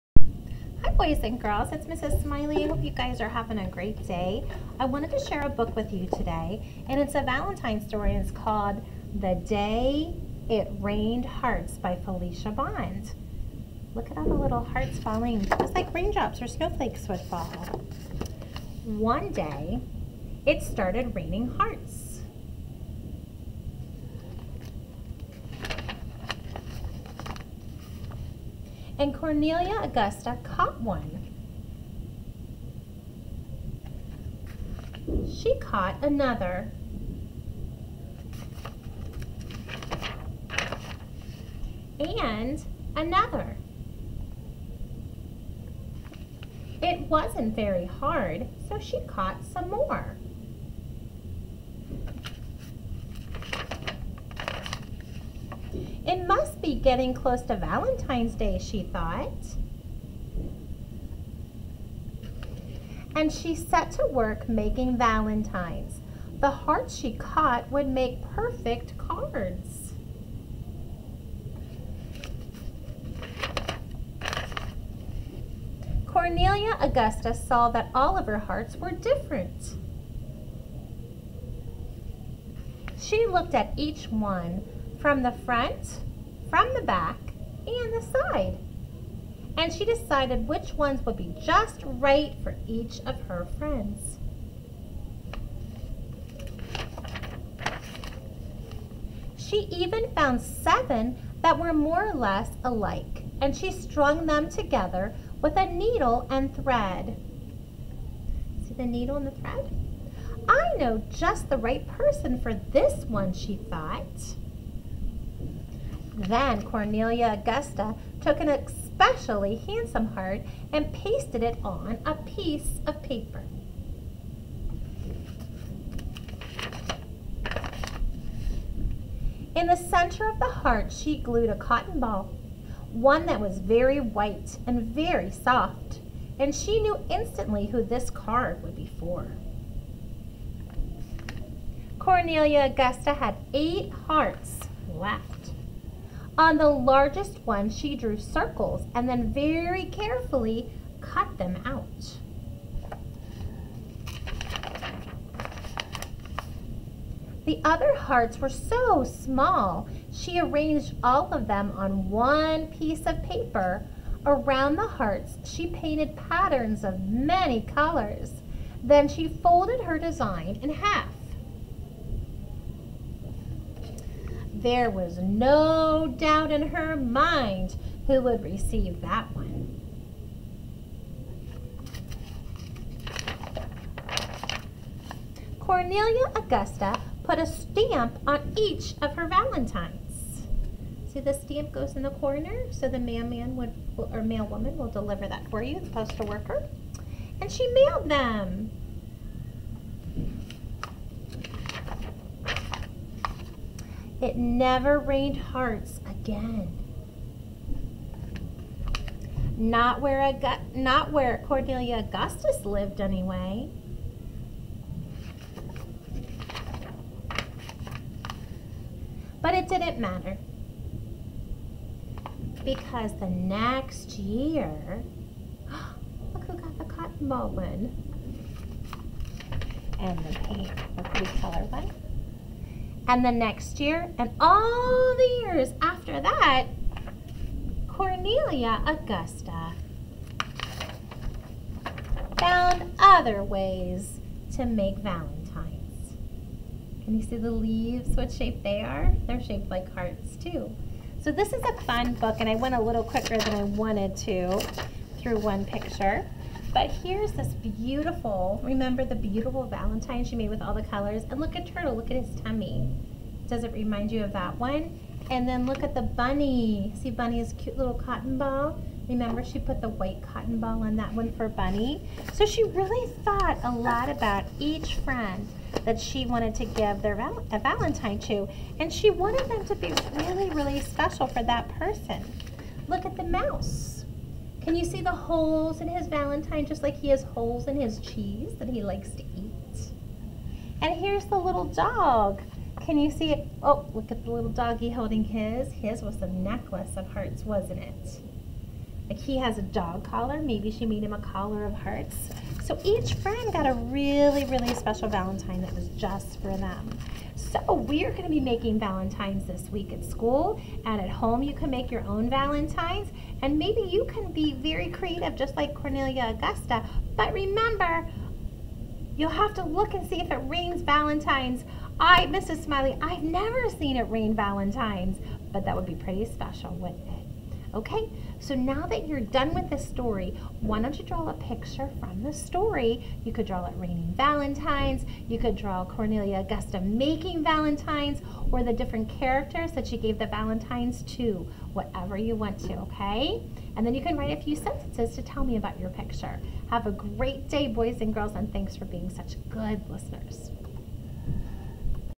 The Day it Rained Hearts
This is a sweet valentine making story.